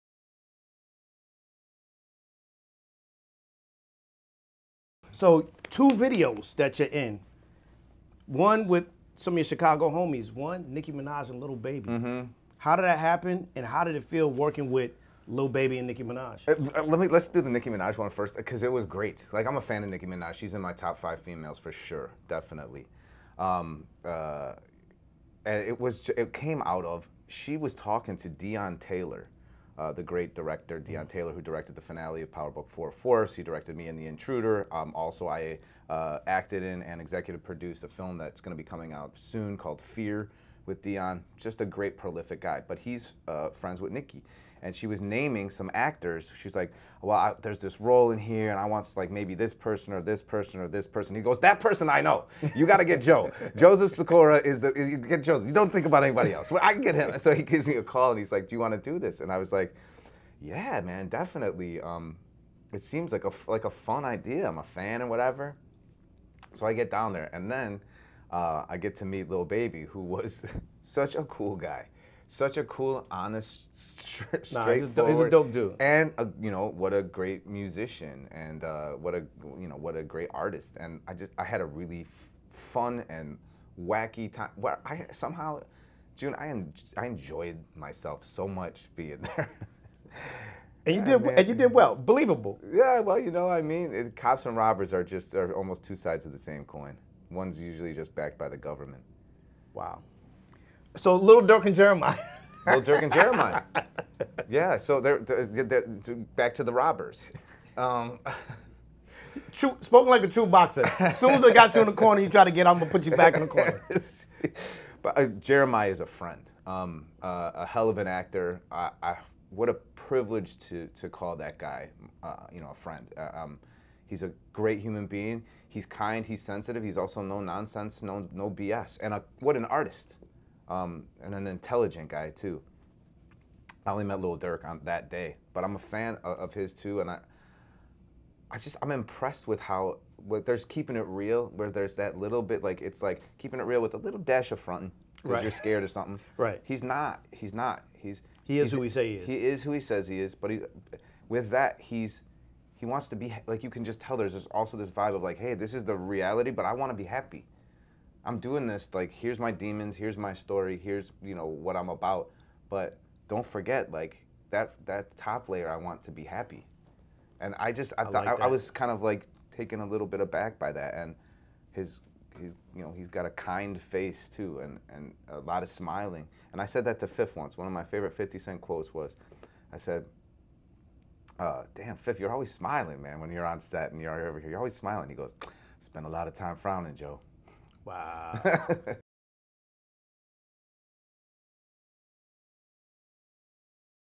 Joseph Sikora talks Working With Nicki Minaj, Lil Baby, Lil Durk & Jeremih
Joseph Sikora on Working With Nicki Minaj, Lil Baby, Lil Durk & Jeremih

Produced by: Jeremy Bettis (@jbettis420)